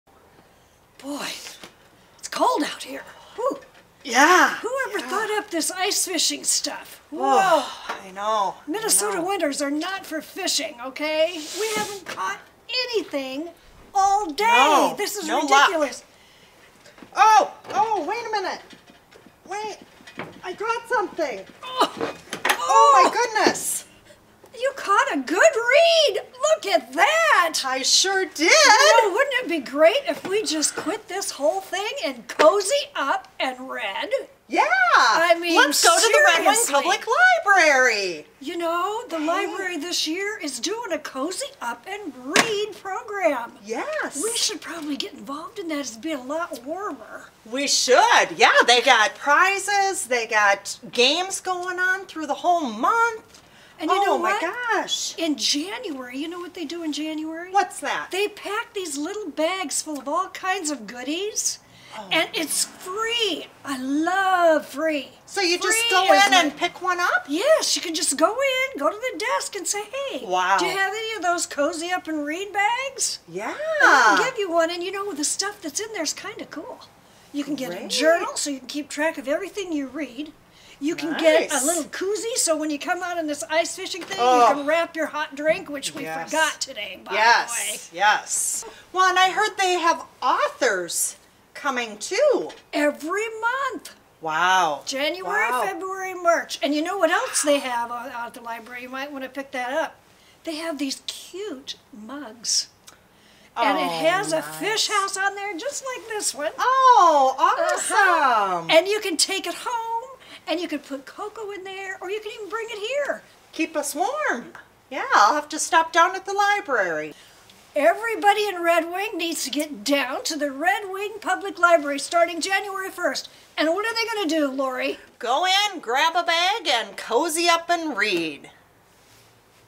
Cozy Up at the Red Wing Public Library
It may be cold outside, but things couldn't be more cozy at Red Wing Public Library.  Sign up for the Cozy Up and Read program beginning January 2019.  You'll get lots of great reading swag, the inside track on author visits, and prizes to boot.